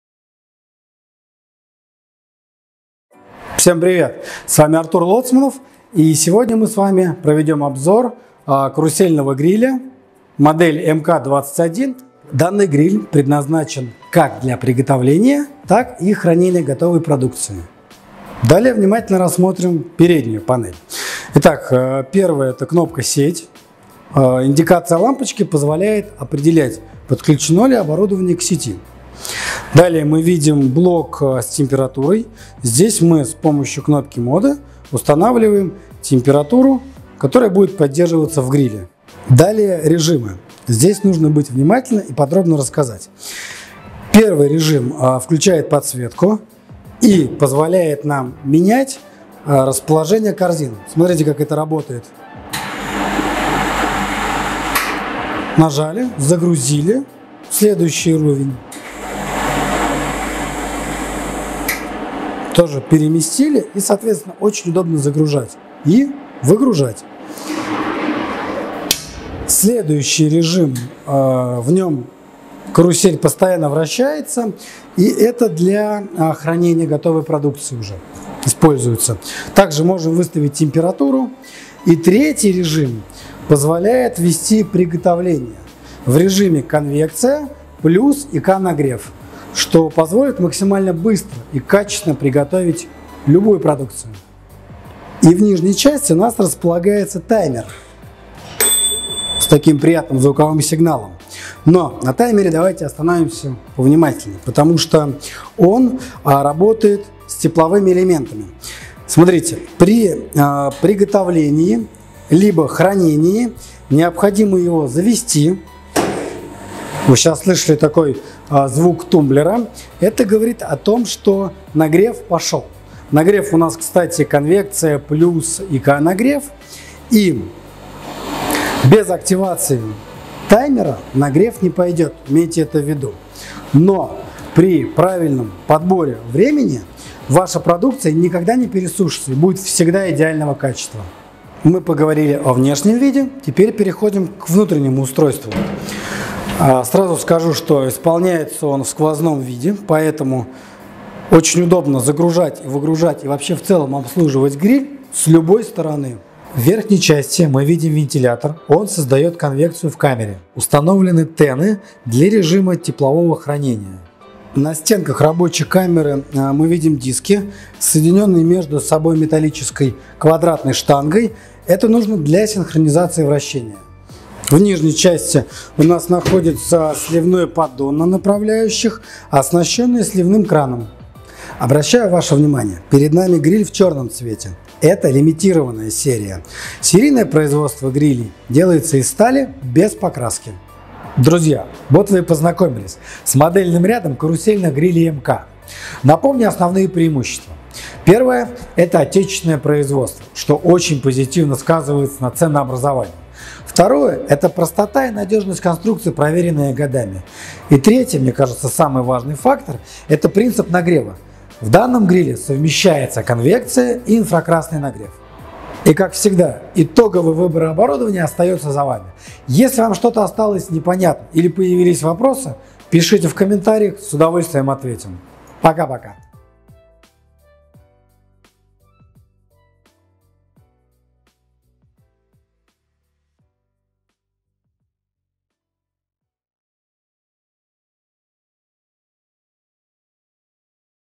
Обзор. Гриль для кур электрический СИКОМ МК-21.
Гриль для кур электрический СИКОМ МК-21. Обзор.

 Электрический гриль серии МК-21 предназначен для приготовления куры-гриль целыми тушками или их частями на специальных люльках.
- напольное и настольное исполнение с вместительными люльками –корзинами для кур весом до 2 кг.
- конвекция горячего воздуха ускоряет приготовление продукта при меньших потерях массы и лучших органолептических свойствах
- сквозные: загрузка и выгрузка продукта может осуществляться с двух сторон
- нагрев камеры комбинированный: конвекция + инфракрасные излучатели (ТЭНы). Возможно одновременное и попеременное использование режимов
- встроенная подсветка
- поддон-жиросборник укомплектован сливной пробкой, упрощающей обслуживание
- встроенный таймер приготовления
- возможно компоновать в колону «гриль-гриль» или «витрина-гриль» . Витрина линейка ВН-4.21, что позволяет на ограниченных площадях создавать полнофункциональные рабочие места гриль-баров.

* В черном цвете лимитированная коллекция.
Возможно исполнение в любом цвете RAL.

Хотите купить оборудование и многое другое?

BusinessRussia 0+